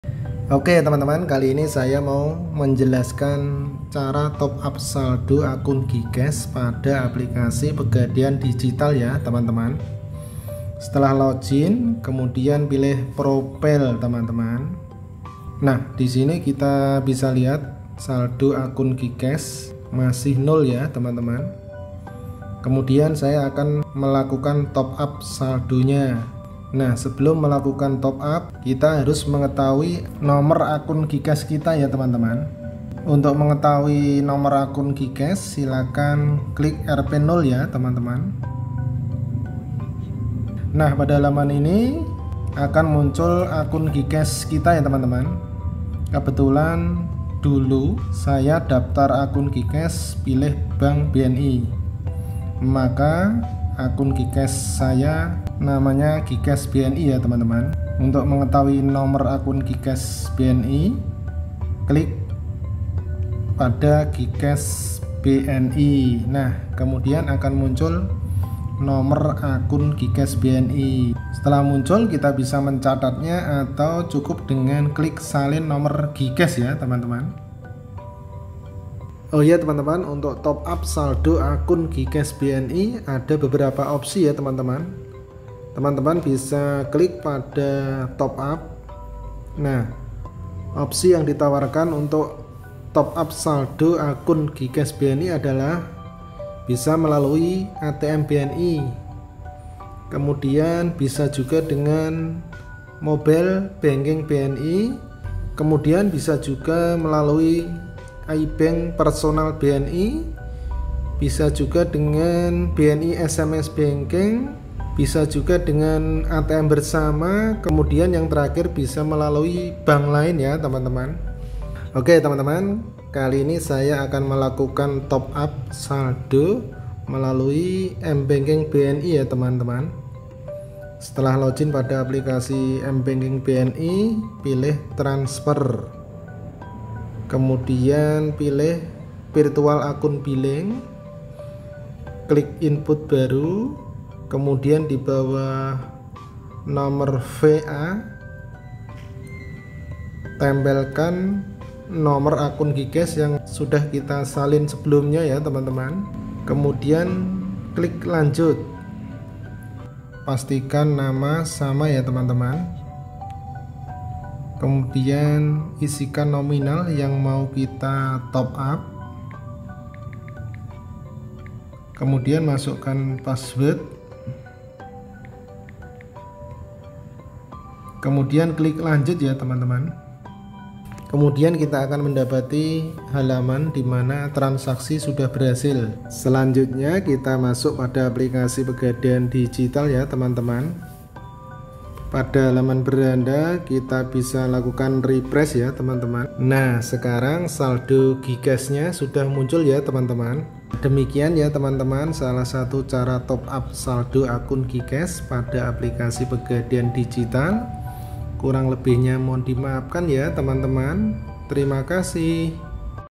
Cara Top Up Saldo G Cash Aplikasi Pegadaian Digital Service
Berikut video tutorial cara top up saldo g cash pada aplikasi pegadaian digital yang bisa digunakan untuk referensi.